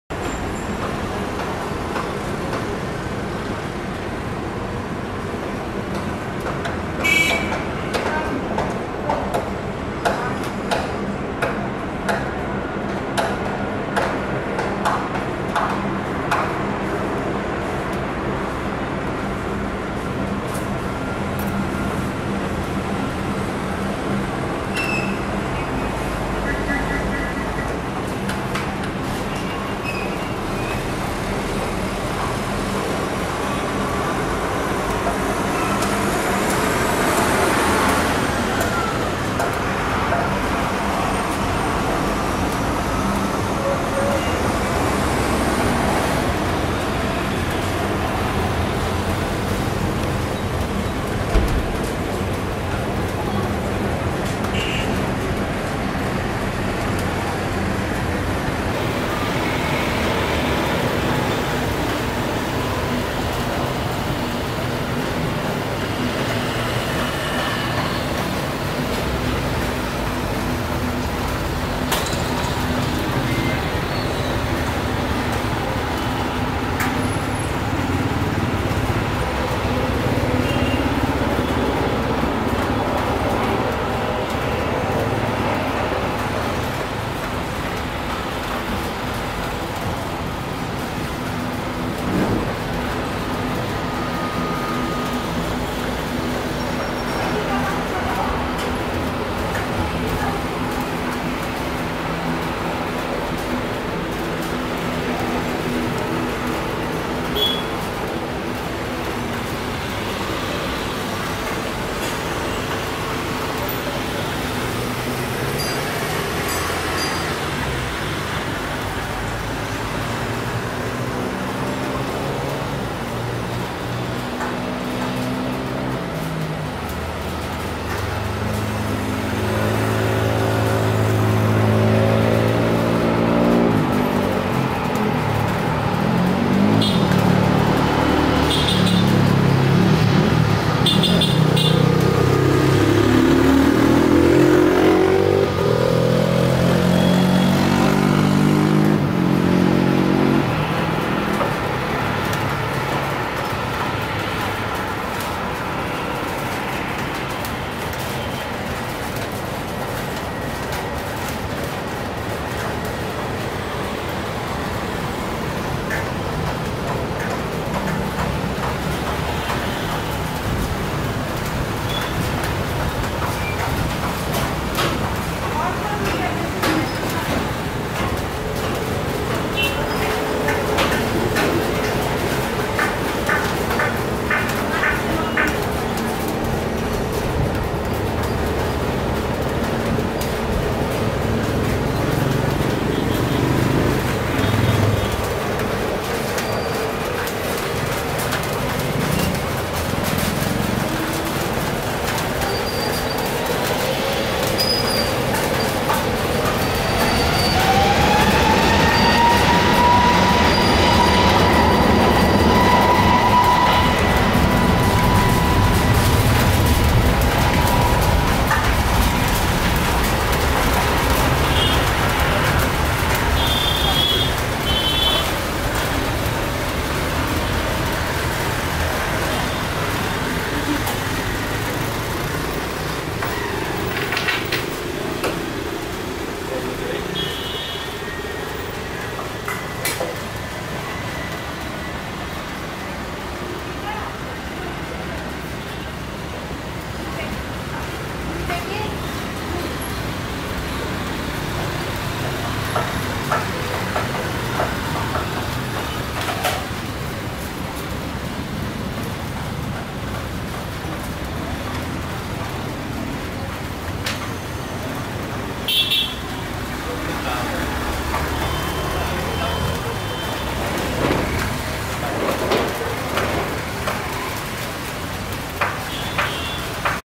Salam Air Airbus A320 Quick takeoff from Tribhuvan Airport after नेपाली सेनाको Augusta AW139 Lands
Do Share and Subsribe AviationNepali Channel for More Aviation related videos.